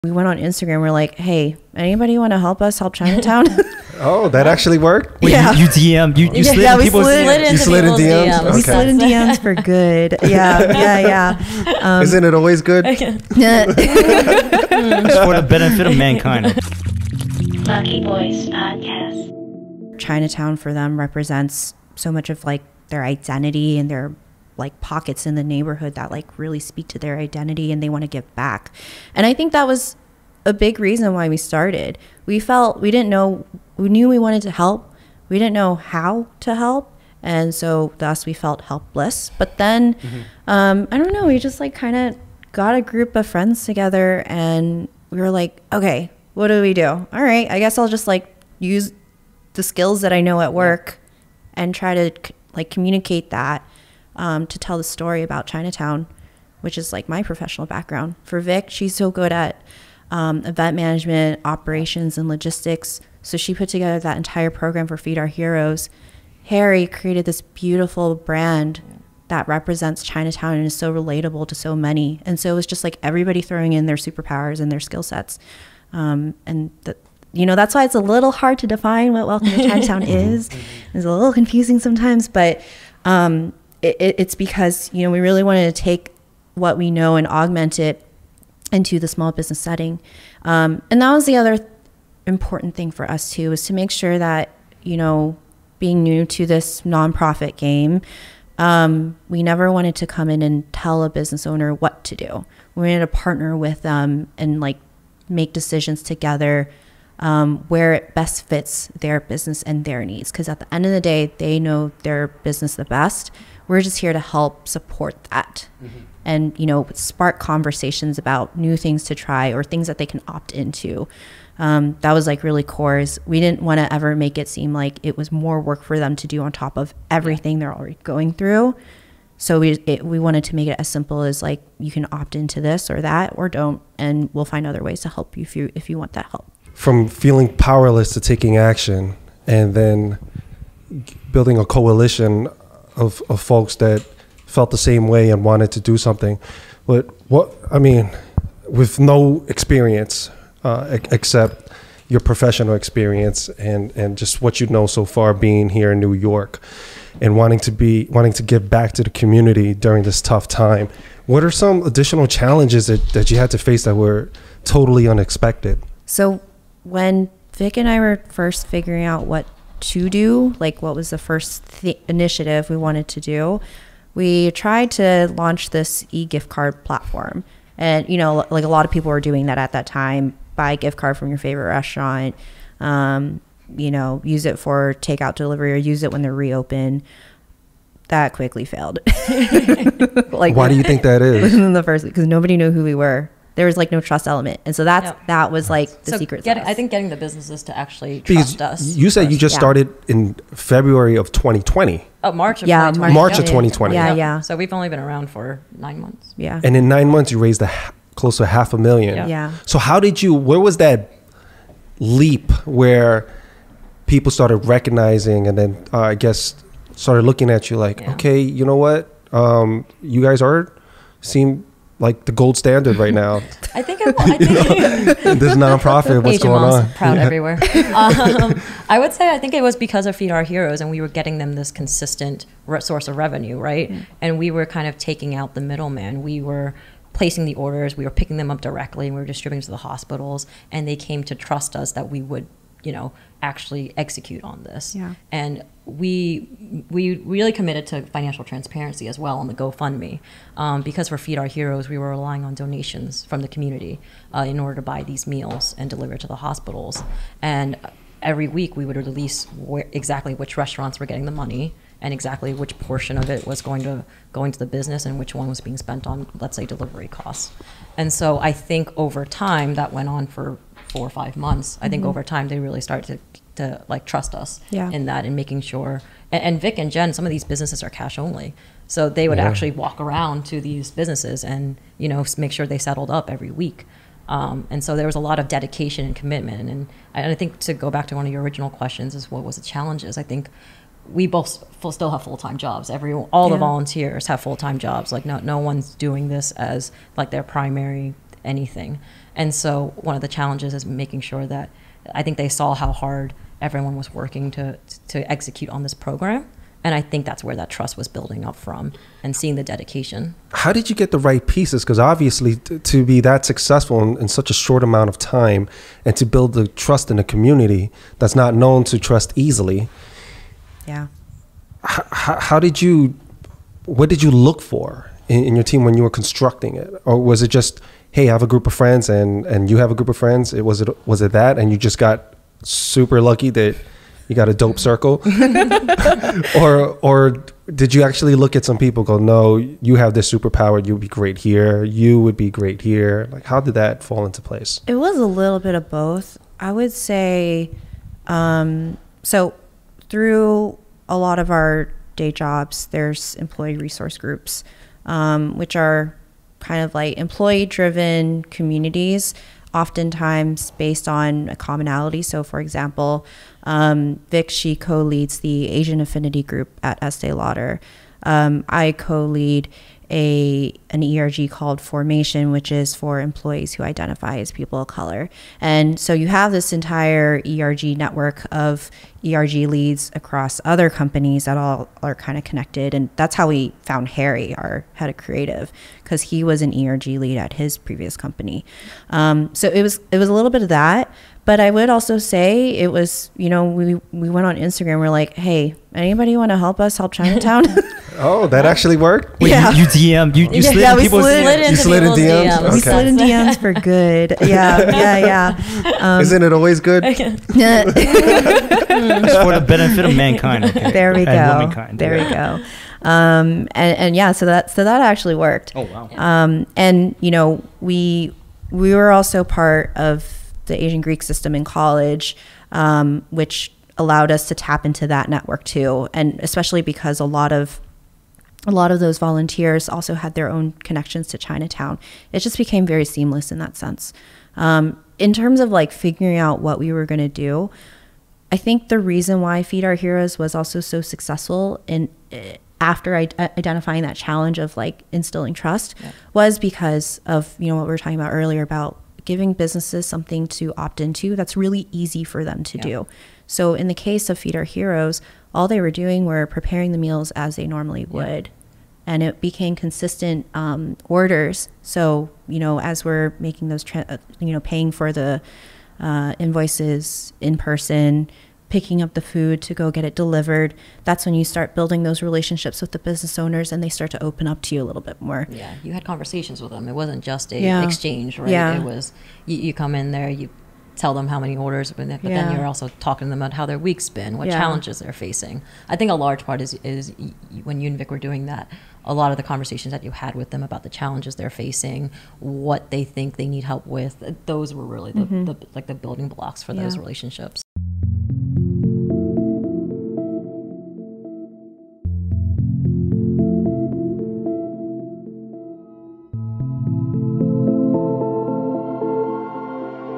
From Feeling Helpless To Helpful: Raising 500k For Struggling Chinatown Businesses | Lucky Boys
Early Access To Full Video Episodes.

Lucky Boys Podcast:
William Hue

Produced By
Old Kid Productions